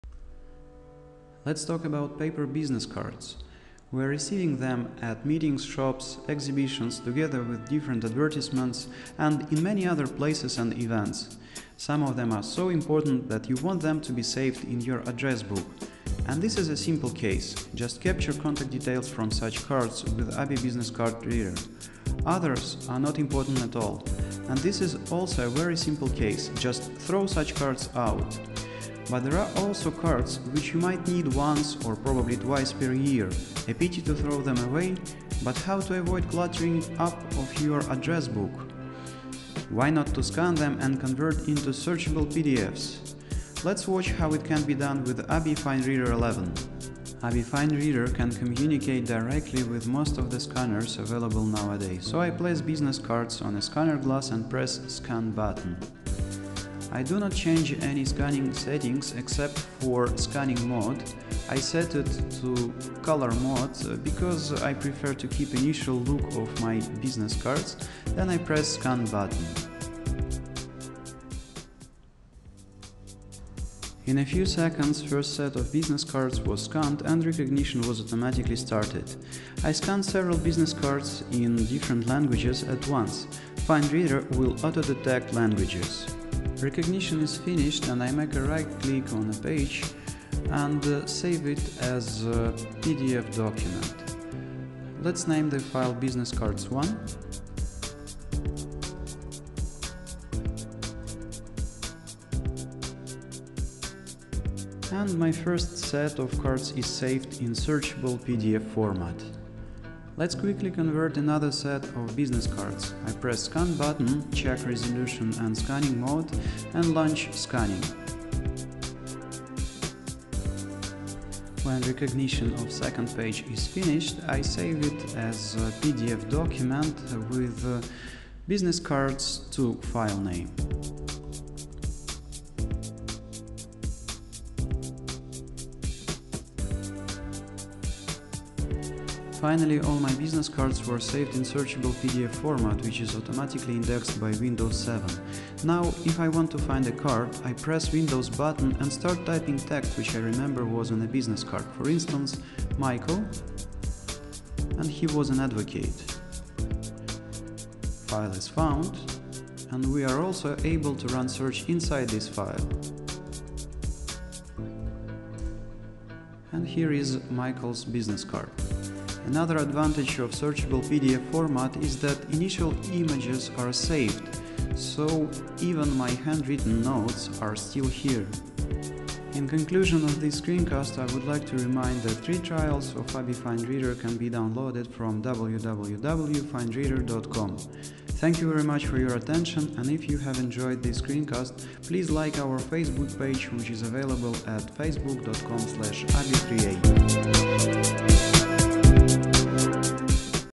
How-to No.12 — Digitize paper business cards without cluttering up your address book.
If you have a lot of business cards which you might need once or twice a year only it is better not to clutter up your address book, but to save these cards in searchable PDF. On the one hand this format allows to keep the original look of the cards, on the other -- to organize full text search. ABBYY FineReader 11 can easily cope with the task of conversion of scanned business cards into searchable PDF. In this screencast it is shown how to
— scan multiple business cards simultaneously
— recognize multilingual business cards
— save recognized cards in searchable pdf
— search for needed pdf file
— search for a business card inside pdf